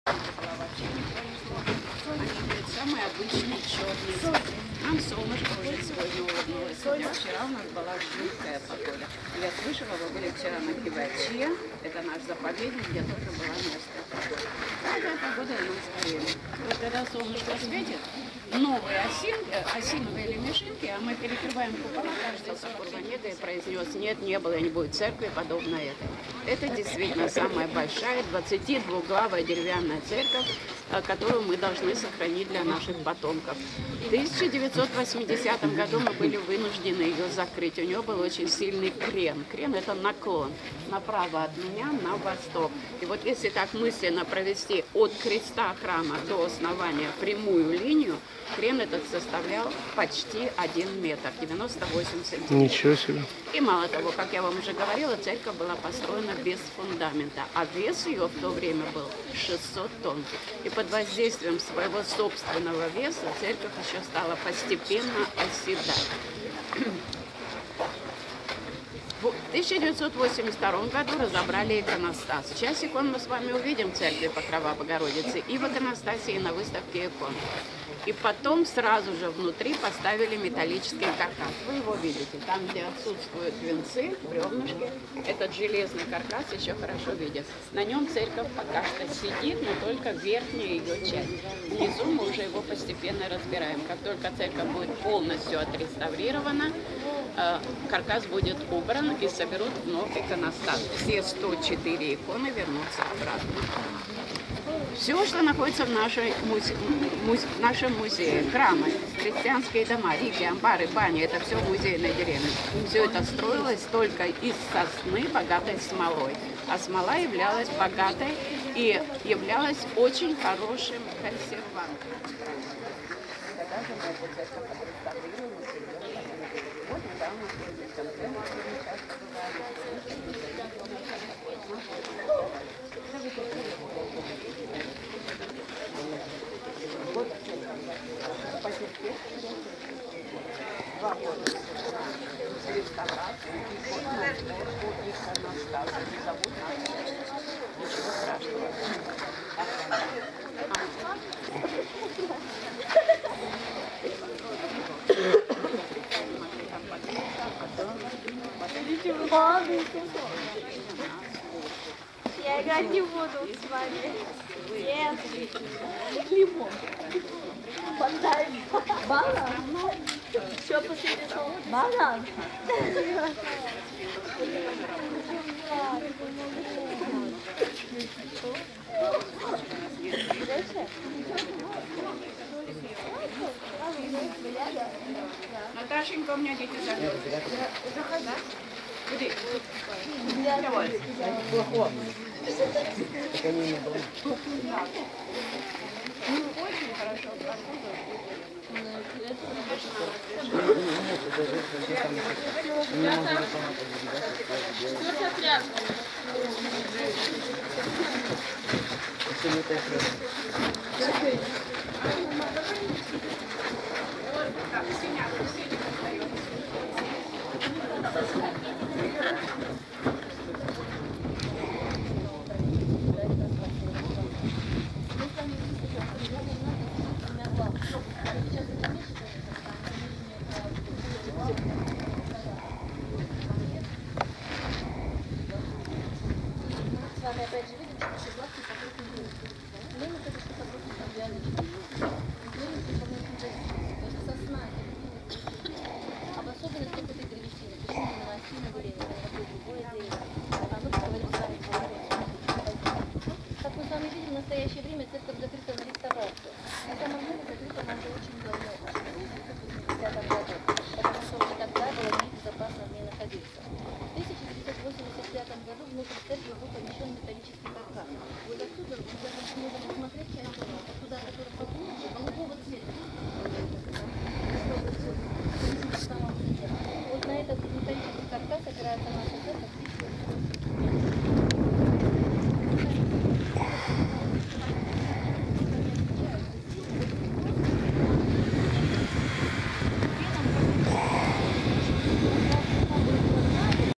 Т/Х "НИКОЛАЙ ГОГОЛЬ" 2015 - Кижи / M/S "N. GOGOL'" - Kizhi (3)
"Жемчужина" древнего, православного, русского, деревянного зодчества - Церковь Преображения Господня.
Церковь покрова Божьей матери.
Шатровая колокольня "Звучание острова".
22 мая 2015 г., остров Кижи, Карелия,
съёмка моя